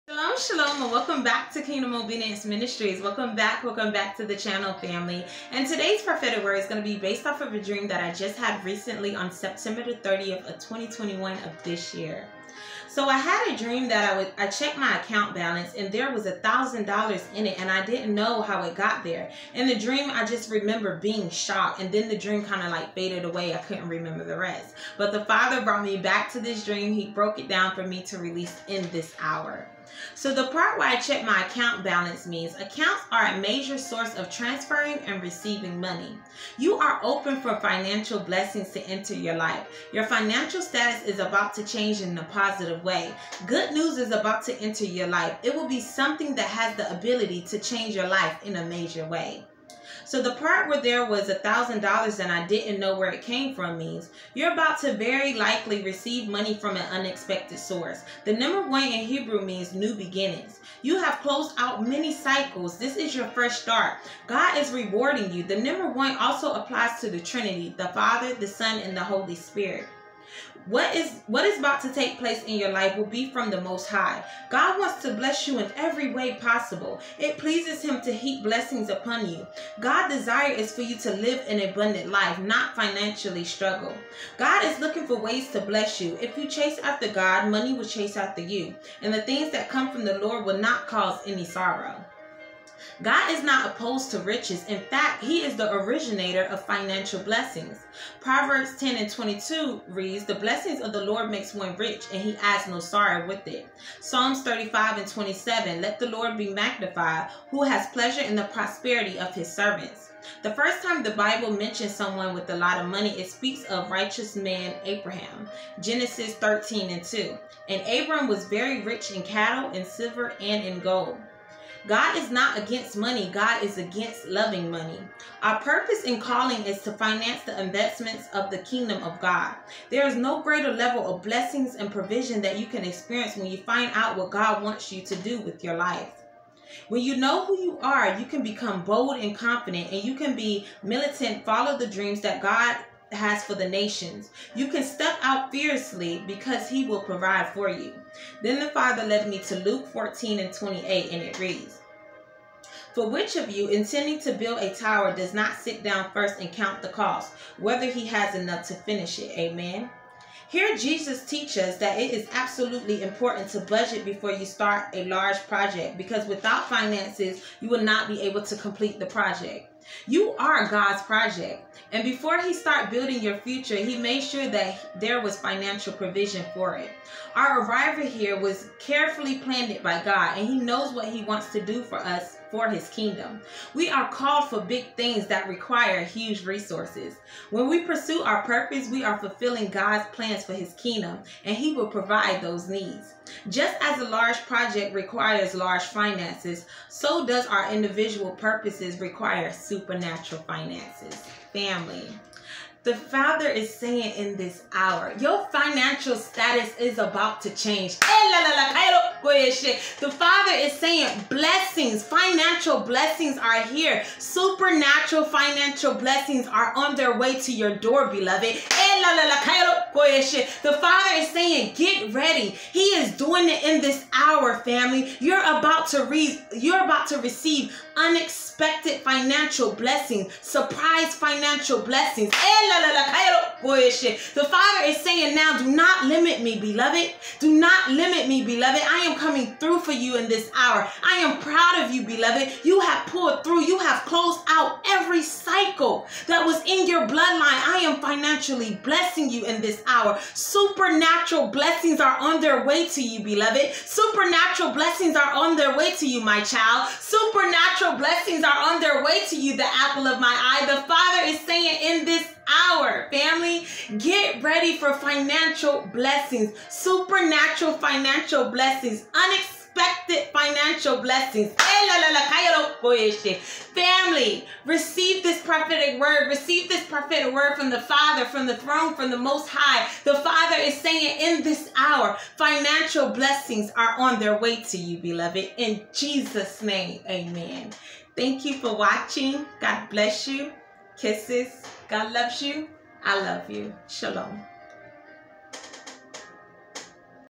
God Said, Your Financial Status Is About To Change❗❗ 🥳 | Prophetic Word
This prophetic word comes from a dream I had on September 30,2021 God is saying in this hour you are open for financial blessings good news is about to enter your life it will be something that has the ability to change your life . You are about to receive a financial blessing from an unexpected source .  I plead the blood of Jesus over this prophetic word in Jesus Name Amen 🙏Contact me: If the holy Spirit leads you to sow into this ministry ~ Cash App: $kingdomobedience PayPal